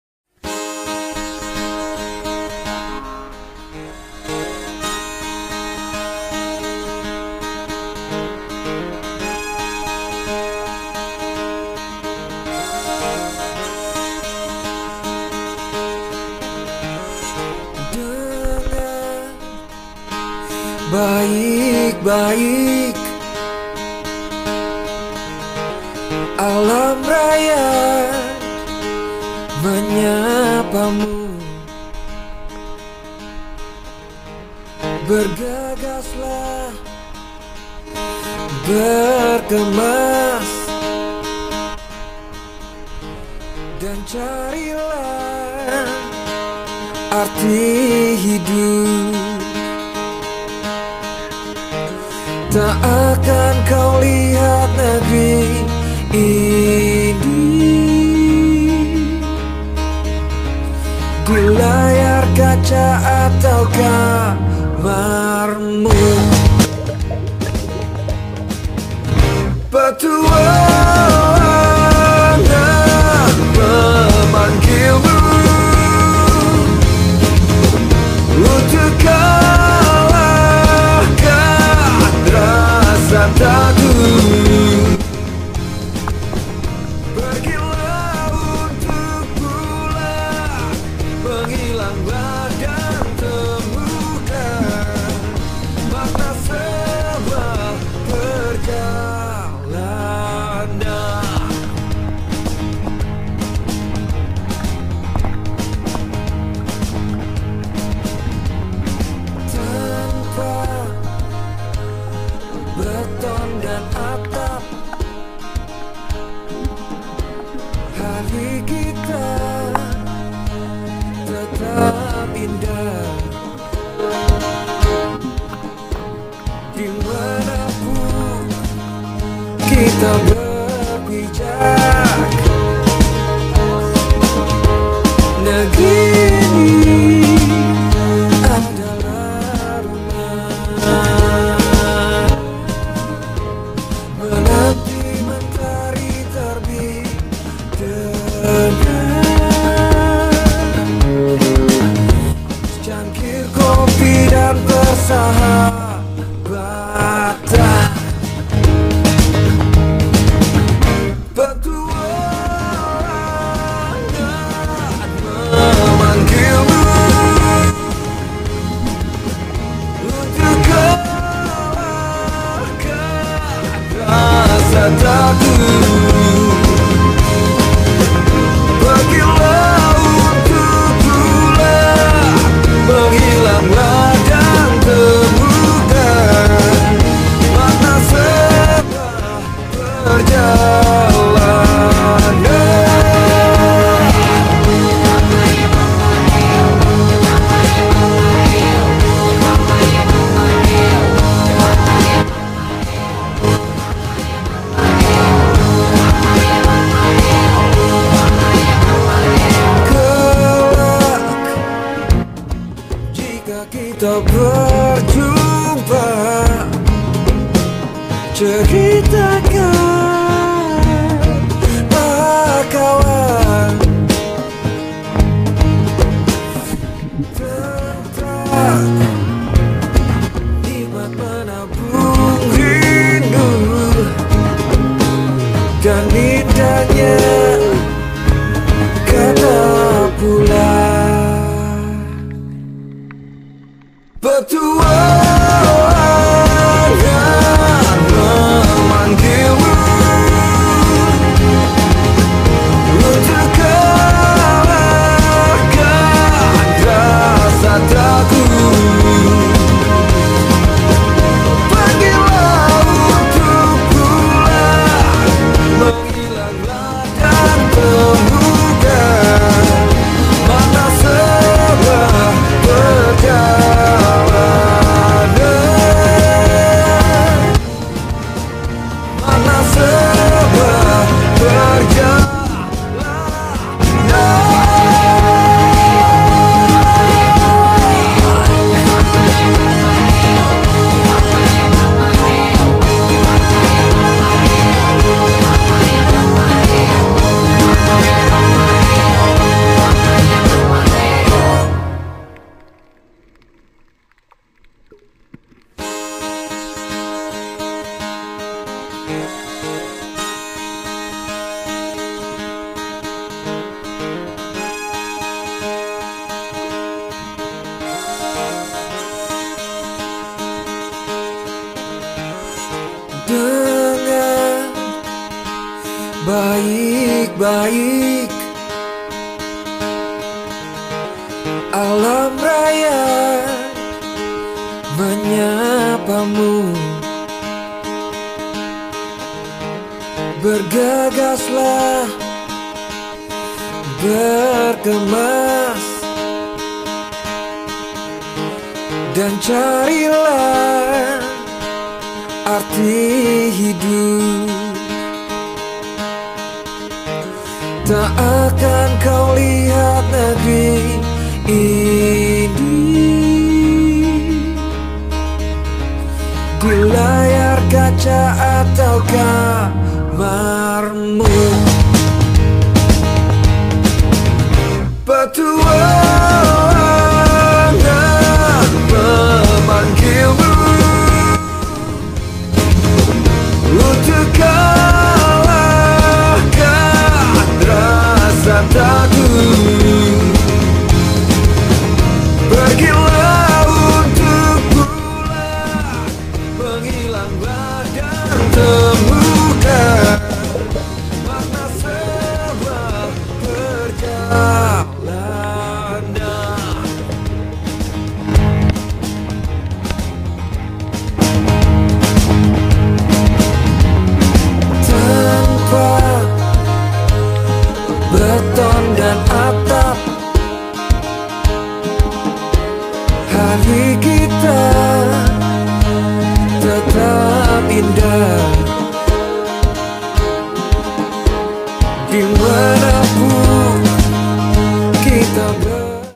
SALAM PETUALANGAN DARI UJUNG INDONESIA
DAMPAK POTENSI WISATA BAHARI DAN LESTARI TRUMBU KARANG SERTA BIOTA MENJADI UNIVERSITY UNTUK MELAKUKAN PENELITIAN2 BAWAH AIR
Created by : Explore Aceh Tamiang X Giraffes story Production